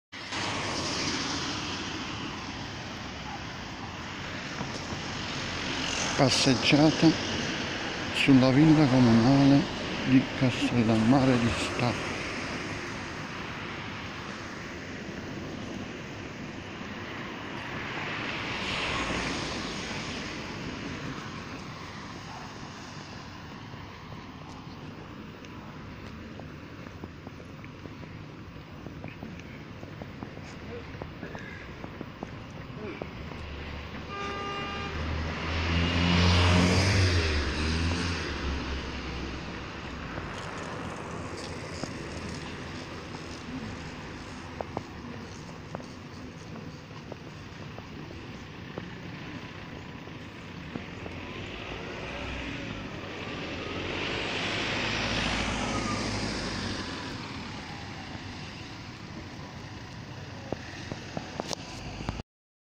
27 agosto 2019
Passeggiata sulla villa comunale di Castellammare di Stabia